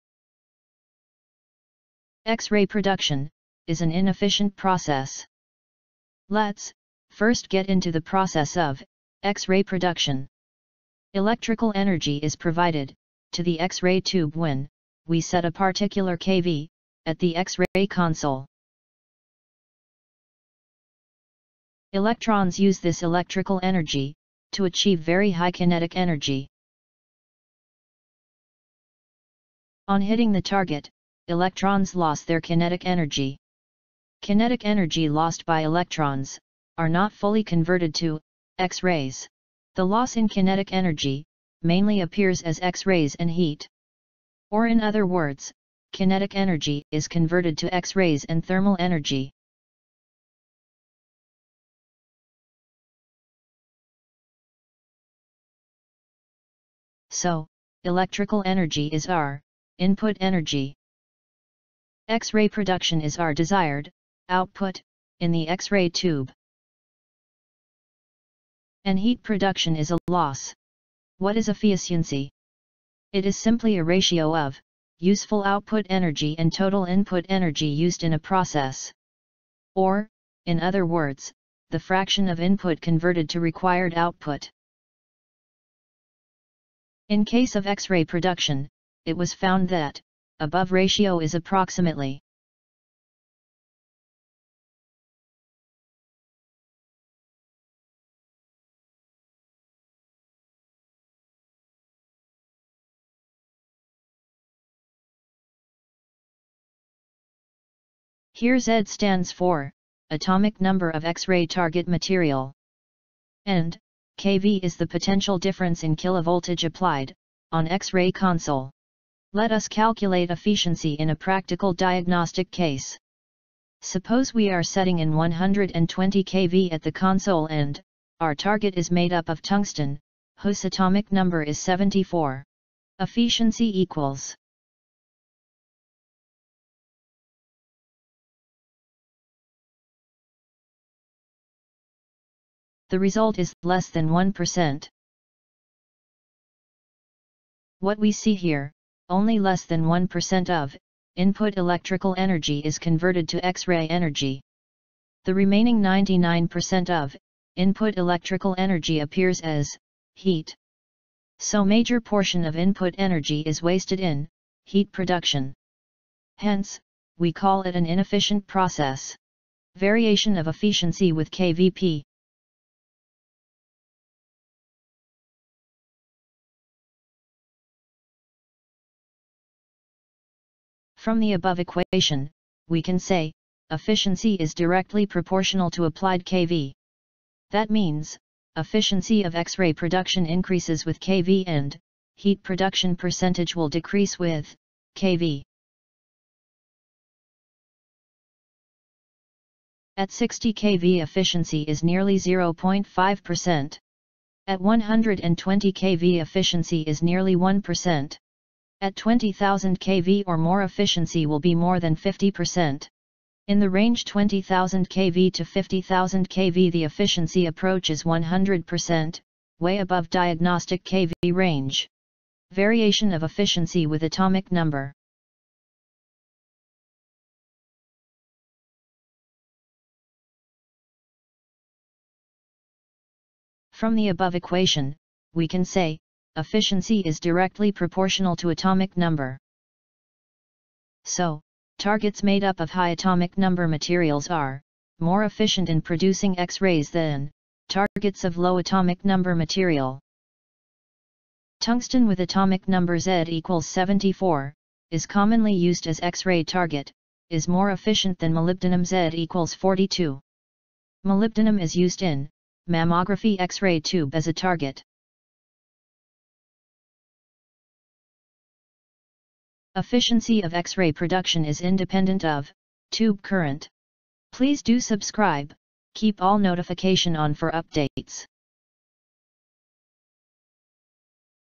efficiency of xray production
in diagnostic Radiology xray production is an inefficient process. Most of input energy is wasted as Heat.